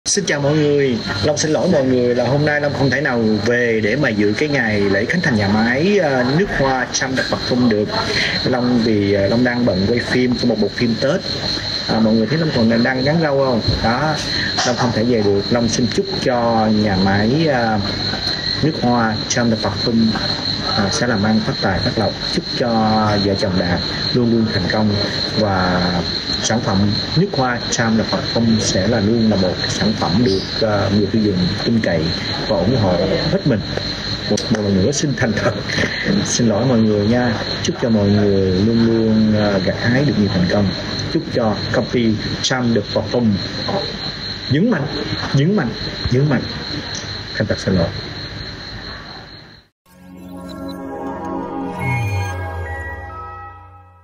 NSƯT Kim Tử Long gửi lời chúc đến Charme Perfume nhân dịp Lễ khánh thành nhà máy sản xuất thứ ba
CÔNG TY CỔ PHẦN CHARME PERFUME
Địa chỉ: 311 Hoàng Văn Thụ, P.2, Q.Tân Bình, TP.HCM